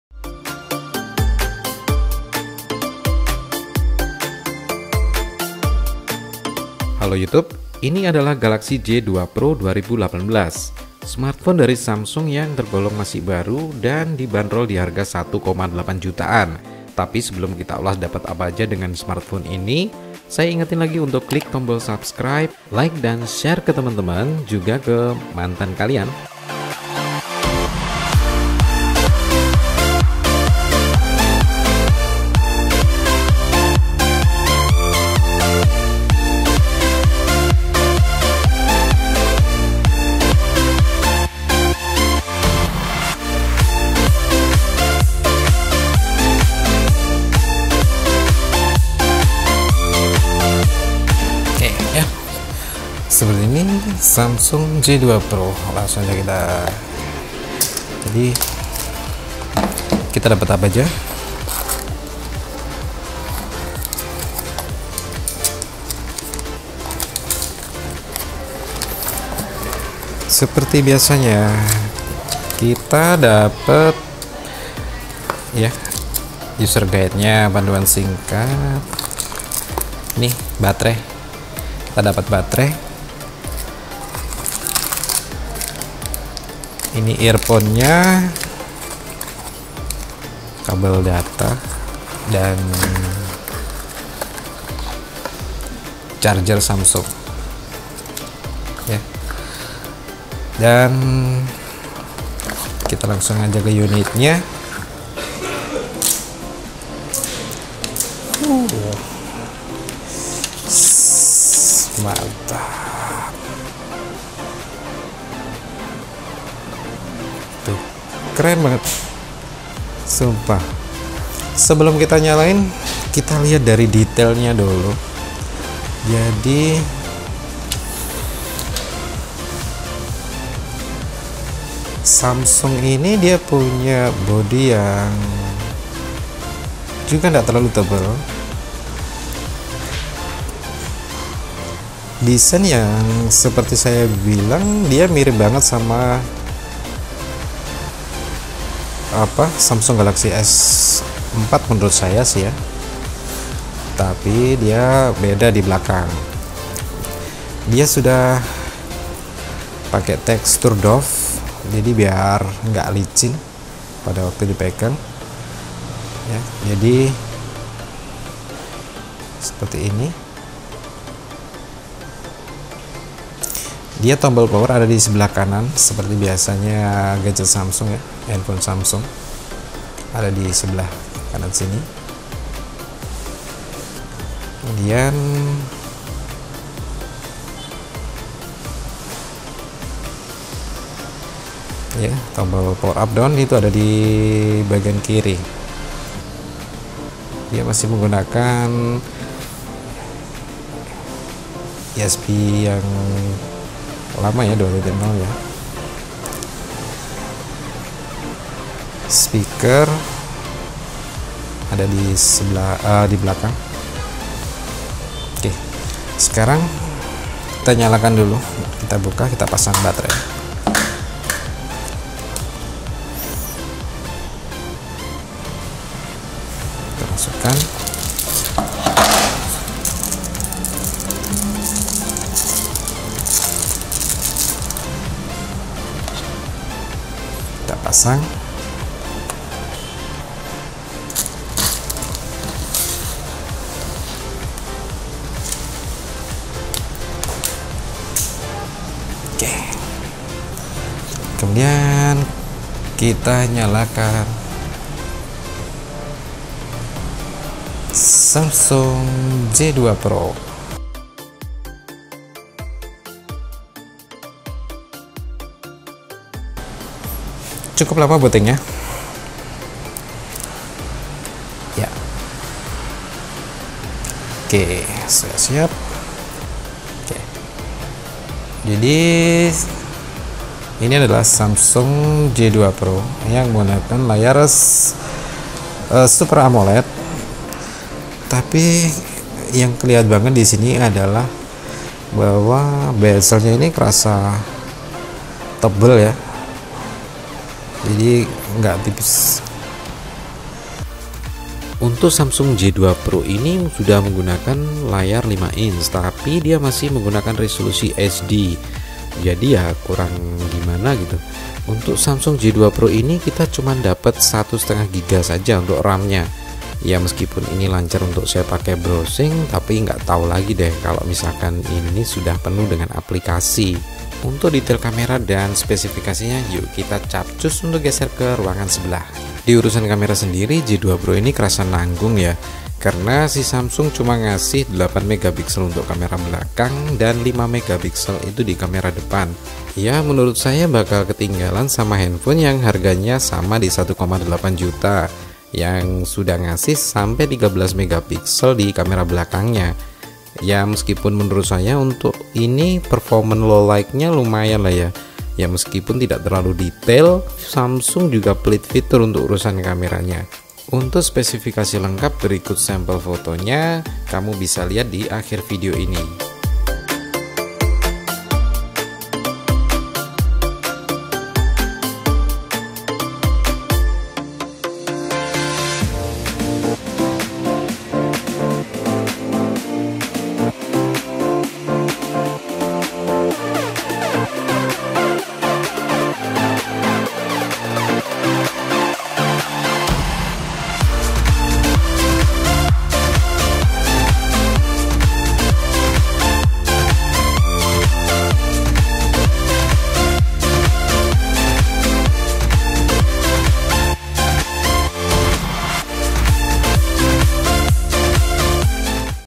UNBOXING DAN REVIEW SAMSUNG GALAXY J2 PRO 2018 INDONESIA
Kali ini dapet jatah J2 Pro untuk kita unboxin dan review , handphone dengan harga dibawa 2 juta tapi masih terasa tanggung banget . Mau tau seperti apa lengkapnya ? budayakan nonton video sampai habis ya